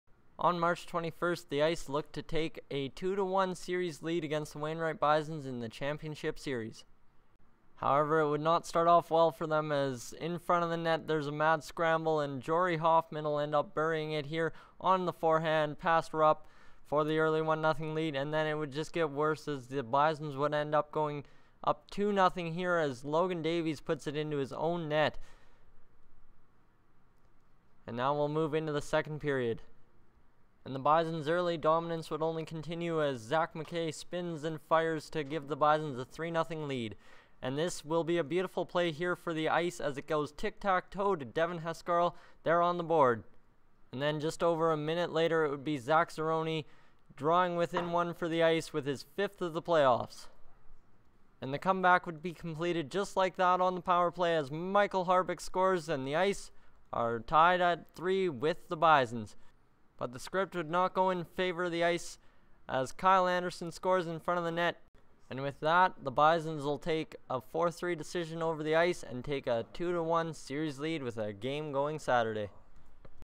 Cold Lake Ice Vs Wainwright Bisons Game 3 NEAJBHL Championship Mar.21/2014
The Wainwright Bisons take Game 3 of the NEAJBHL championship series against your Cold Lake Ice. It was a close one. Tyson Dolynny commentates.